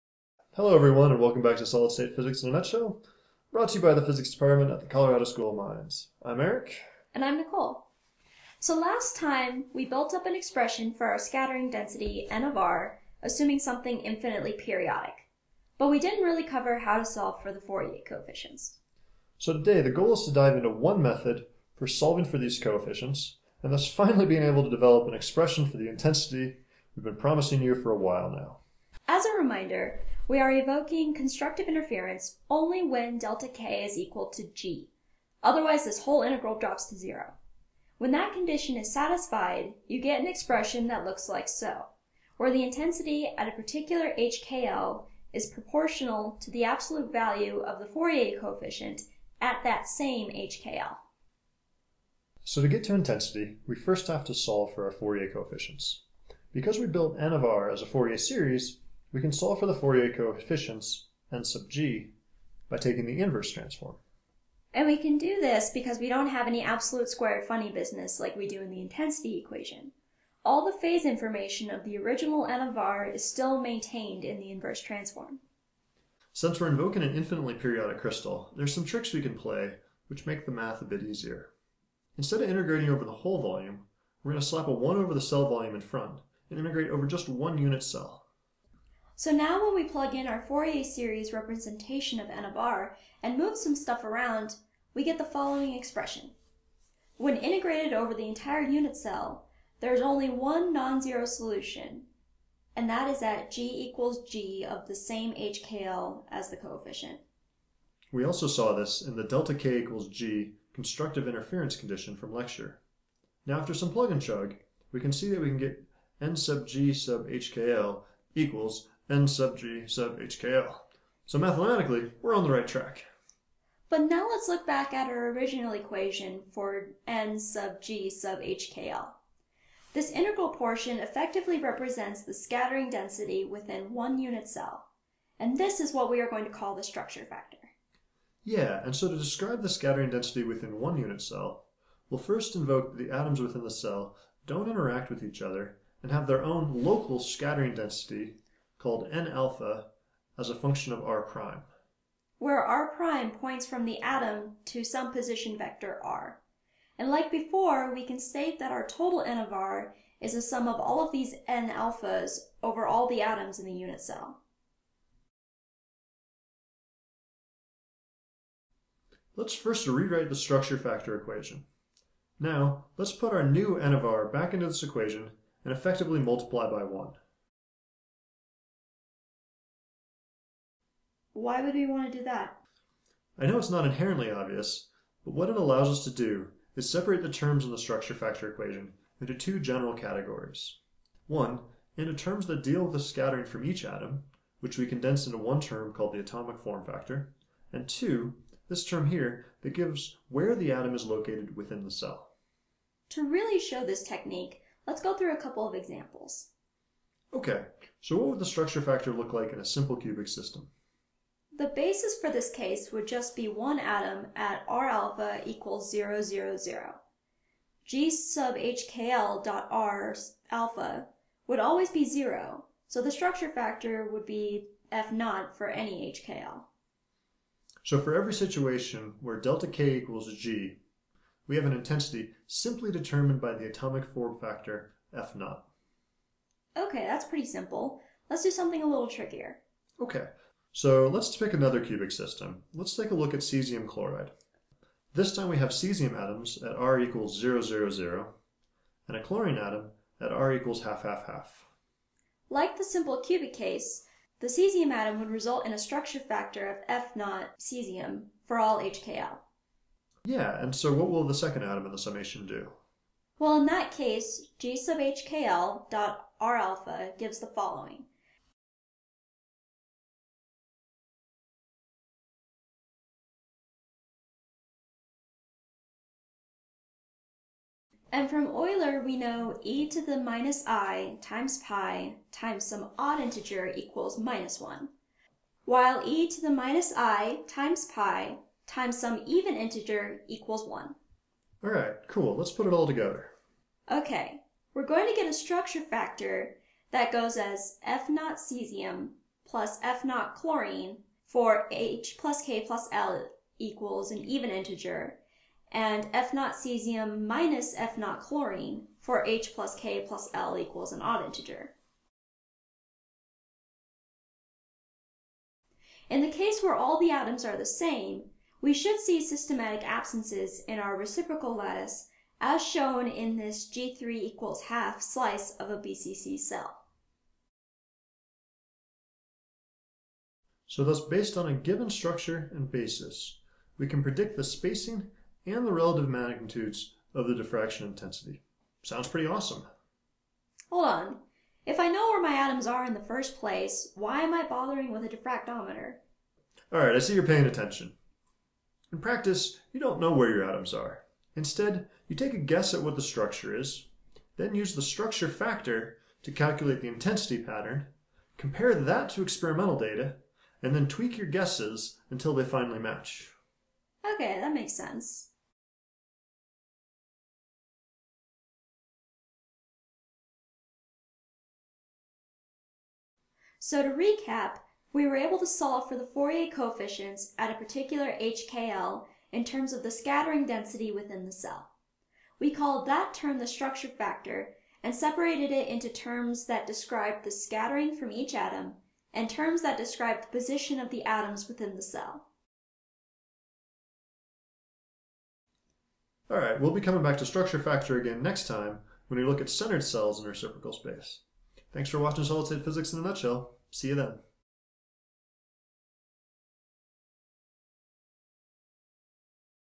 Solid State Physics in a Nutshell: Week 3.4 Structure factor
First semester solid state physics short videos produced by the Colorado School of Mines.  Referenced to Kittel's 8th edition.